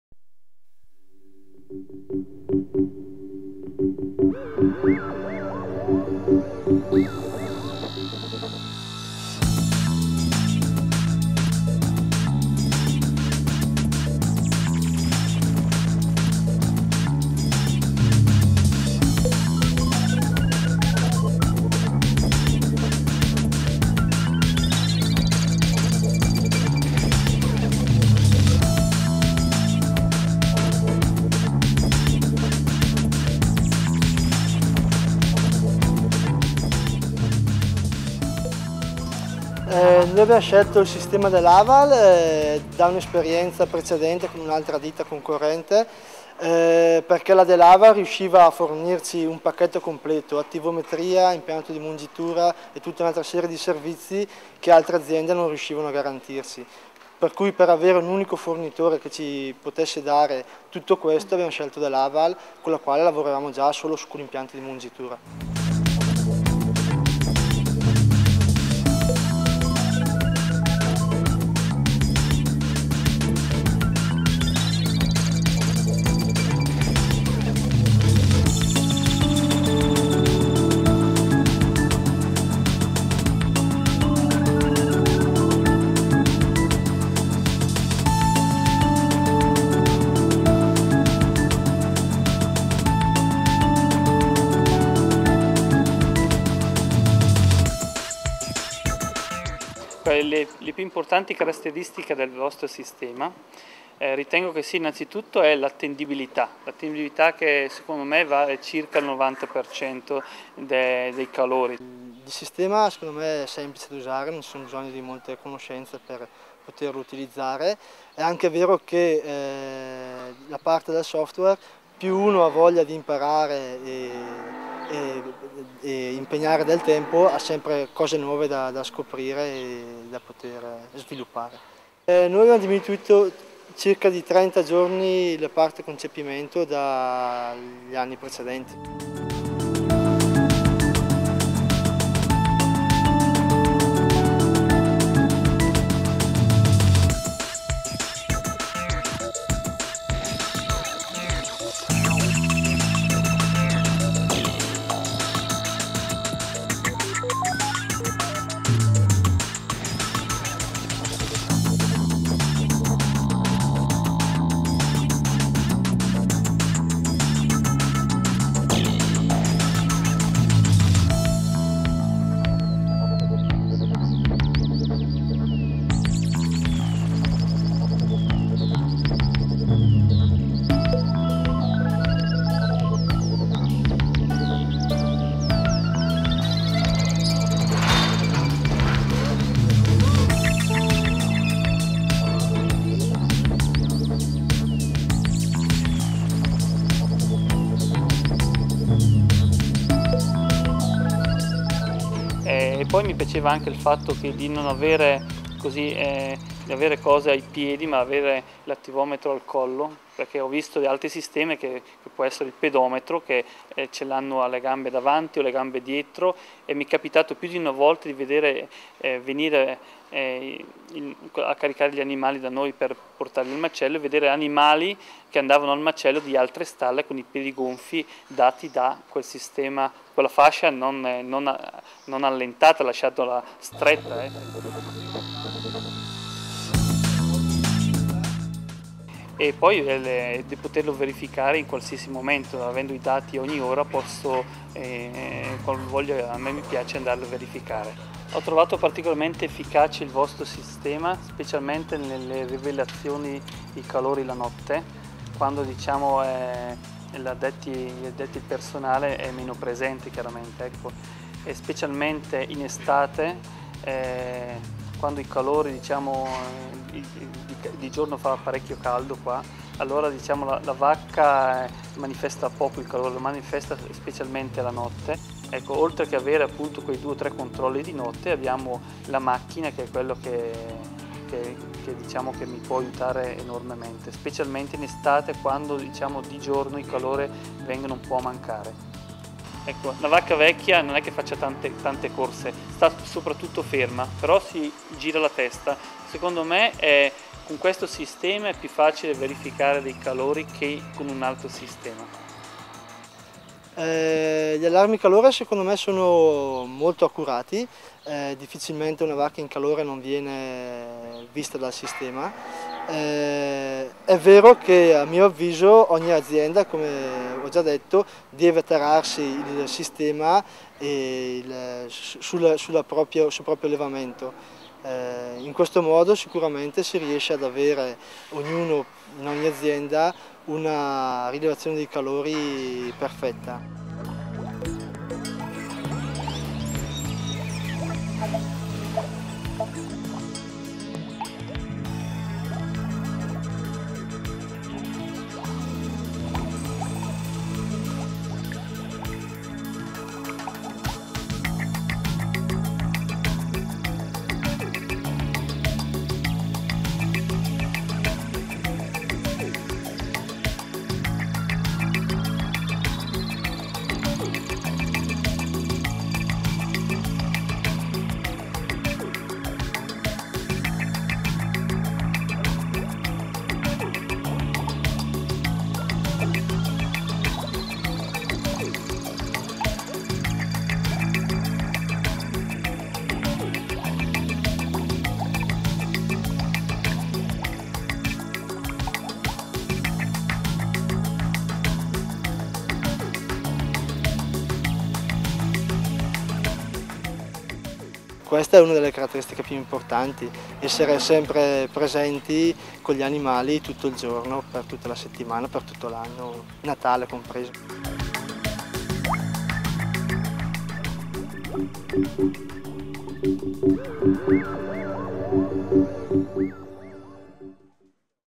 DeLaval Activity Meter | Italy | DeLaval Automated Milking Solutions
DeLaval activity meter system is designed to give the highest hit rate for finding cows in heat, together with excellent cow comfort and 24 hour surveillance.

Finding a heat is not just the basis for all dairy operations -- it is also time consuming and costly. DeLaval activity meter system is proven to generate an unsurpassable 95 percent heat hit rate. This leads to lower breeding costs due to reduced days open and fewer services per conception. It also increases conceptions on animals with silent heats.